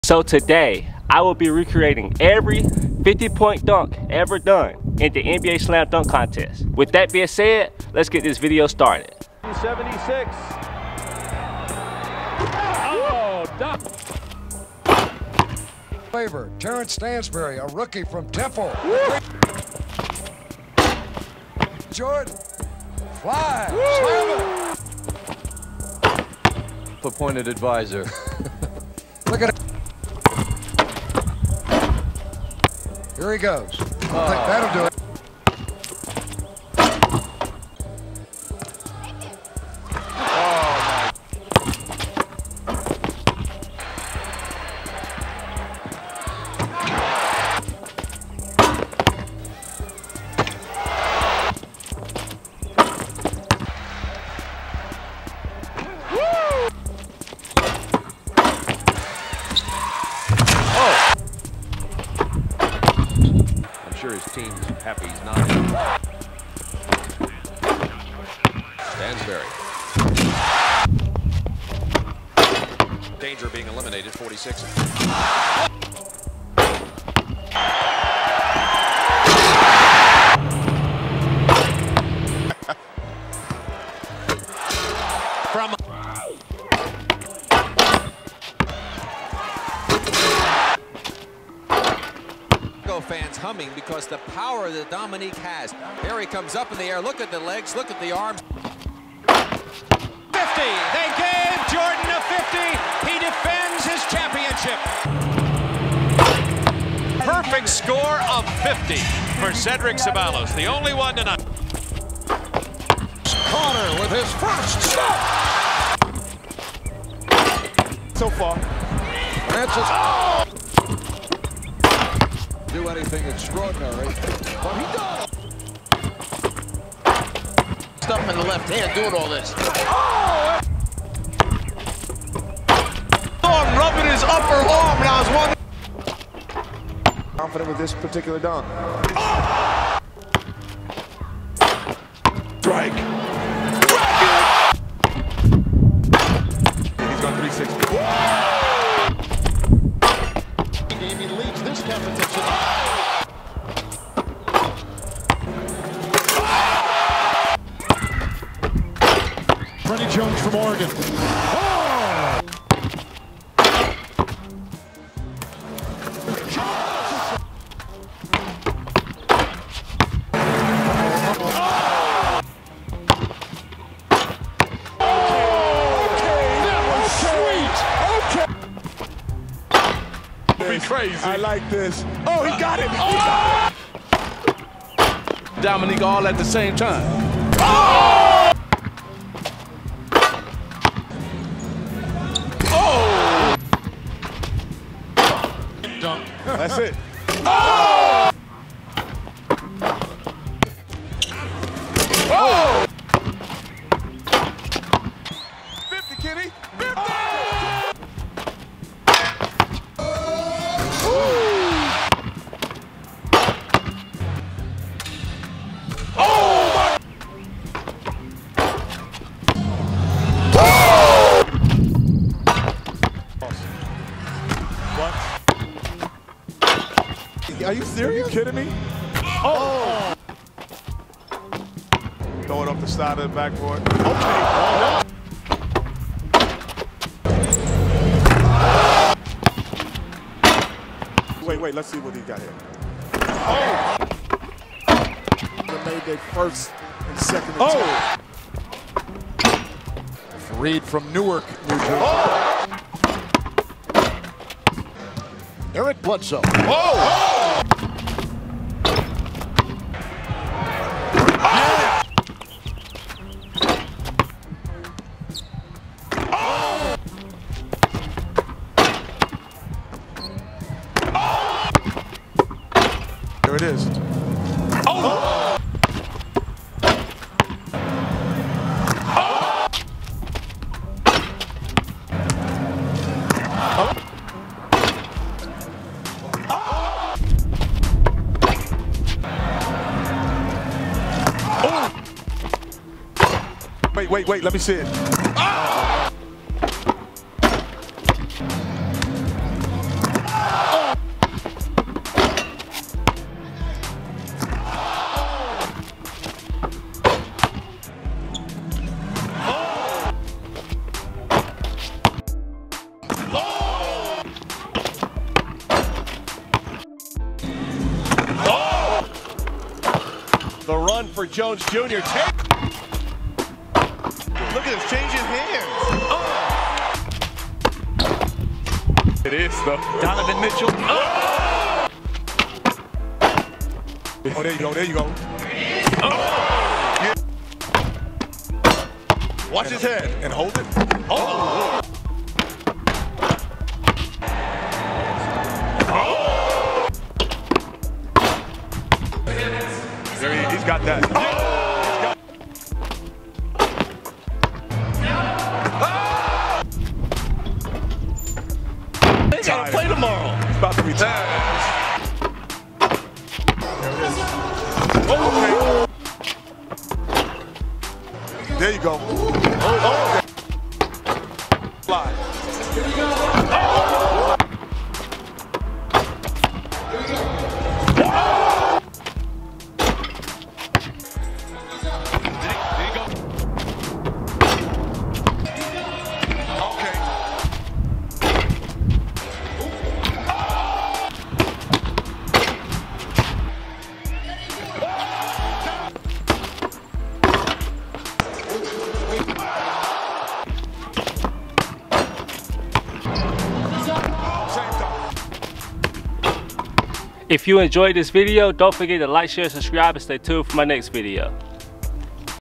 Recreating The Greatest NBA "DUNK CONTEST EVER" Dunks Of All Time
In this video I will be recreating the greatest NBA "DUNK CONTEST EVER" dunks of all time!